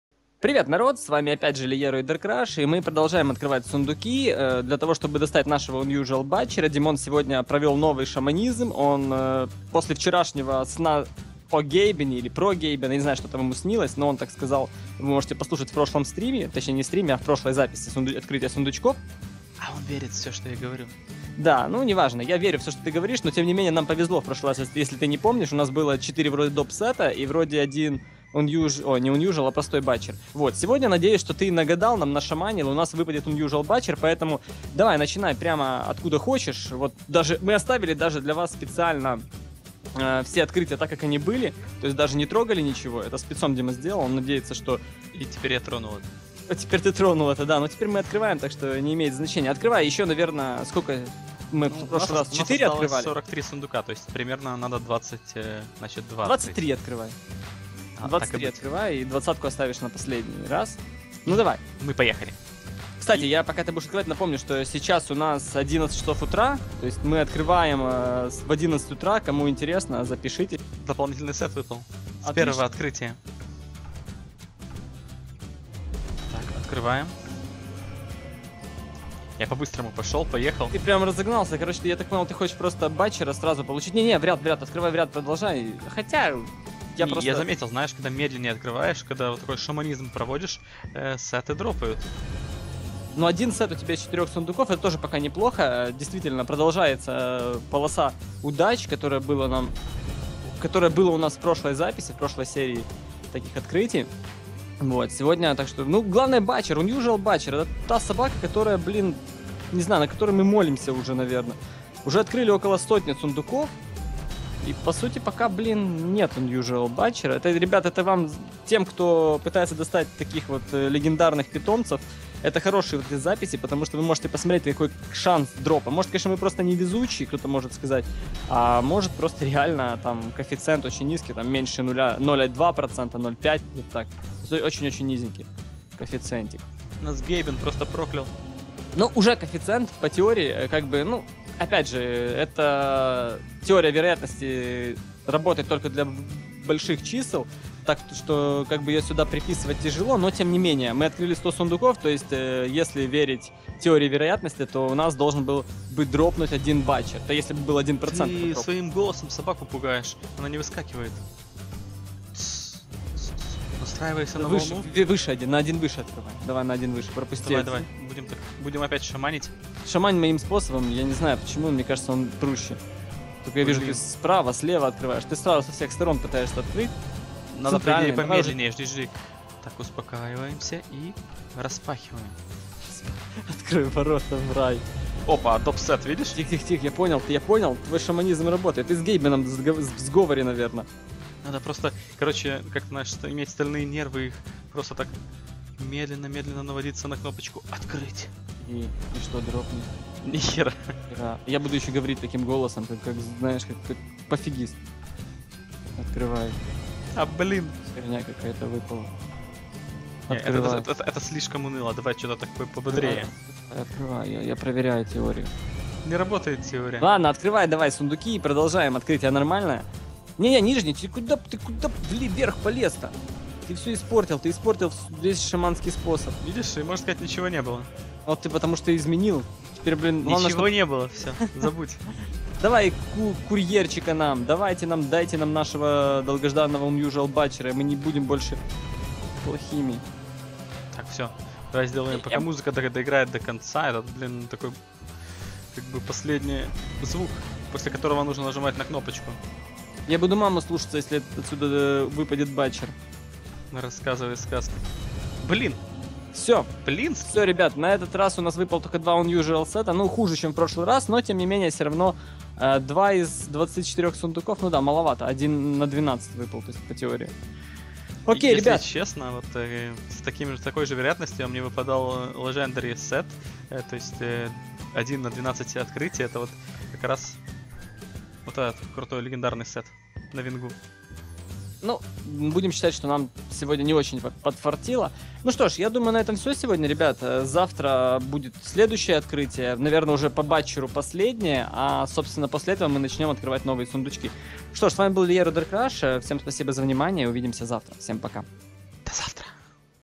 Открываем 139 проклятых Bottled Cloud (6-часть)
Вот Liero и Darkcrash добрались до предпоследнего открытия 139 сундуков. Посмотрим, что им подкинет владыка Гейбен.